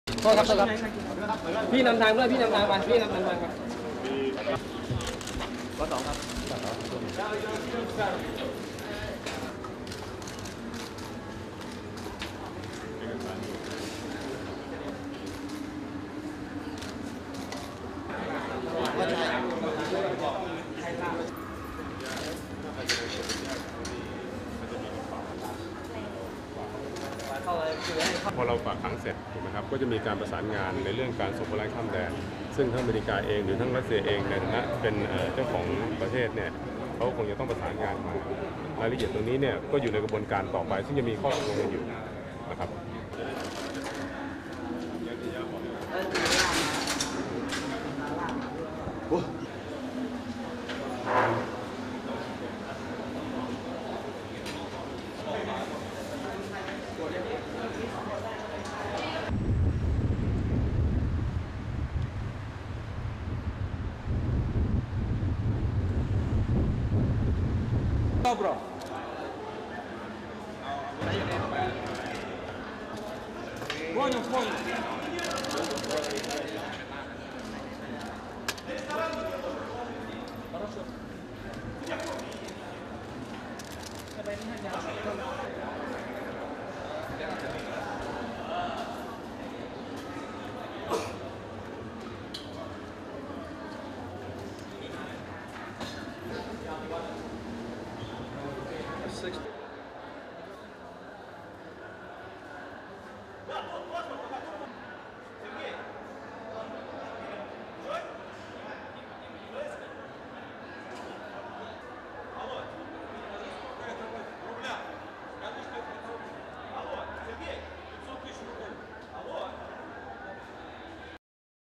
WRAP Russian arrested in arms investigation appears in court ADDS more
1. Various of suspected Russian arms dealer Viktor Bout being taken by Thai police into court
2. Bout being frisked by Thai authorities
3. Mid of Thai police
4. Wide of Bout being frisked
5. Close-up of Police Colonel Kittisak Sukhawattanakul, Superintendent, Crime Suppression Division:
6. SOUNDBITE (Thai) Police Colonel Kittisak Sukhawattanakul, Superintendent, Crime Suppression Division:
"After we finish putting him in jail, there will be a cooperation on extradition (to) the USA or Russia who are originally the owners of this case. All about this detail is in the next process that we have to negotiate."
7. Wide of Bout walking into holding cell
8. View through window of Viktor laughing
9. Officials outside holding cell
10. Exterior of criminal court
++SHOTS 11-16 AND 18 FILMED THROUGH METAL PARTITION++
11. Close-up of Bout
12. Thai police giving Bout piece of paper to sign
13. Close-up of Bout's hand signing,  tilt up to Bout's face, zoom out to Bout walking to desk for finger print
14. Close-up of his thumb being printed on piece of paper
15. Mid of court officials
16. Bout carrying his luggage and walking away
17. Camera with screen showing Bout behind bars
18. Various of Bout standing behind bars
STORYLINE
Thai police received court permission on Saturday to keep in custody an alleged Russian arms dealer who was arrested in a sting operation that lured him to Bangkok from his home in Moscow.
Viktor Bout, 41, was detained Thursday at a hotel in Bangkok, where he had come - according to US officials - to finalise a deal to sell and transport portable surface-to-air missiles and other weapons to men he believed were leftist guerrillas from Colombia.
But the multimillion dollar deal he thought he had with the Colombian rebels was really the culmination of an elaborate four-month sting operation concocted by the US Drug Enforcement Administration, and his customers turned out to be undercover US agents, officials said on Friday.
Bangkok criminal court on Saturday granted police permission to hold him while investigation continues.
Thailand was holding him on charges of engaging with and supporting militant groups.
Suspects may be held for up to 84 days without being formally charged for trial.
A tug of war may develop over who gets to try Bout.
Thai and US authorities both said Friday they could file charges against Bout, and a war crimes prosecutor also expressed a desire to try him for fuelling African civil wars.
Handcuffed and expressionless, the burly Russian was paraded before journalists on Friday at a Thai police news conference but refused to answer questions.
The US has been seeking Bout's extradition, but for now he will remain in Thailand, where authorities are investigating if he used the country as a base to negotiate a weapons deal, Thai police said.
"After we finish putting him in jail, there will be a cooperation on extradition (to) the USA or Russia who are originally the owners of this case. All about this detail is in the next process that we have to negotiate," Police Colonel Kittisak Sukhawattanakul said.
Bout would face 10 years' imprisonment on the Thai charges and 15 years in the US.
Regarded as one of the world's most wanted arms traffickers, Bout's alleged list of customers since the early 1990s includes African dictators and warlords, including former Liberian President Charles Taylor, Libyan leader Moammar Gadhafi and both sides of the civil war in Angola.
He was generally believed to have been a model for the arms dealer portrayed by Nicolas Cage in the 2005 movie "Lord of War."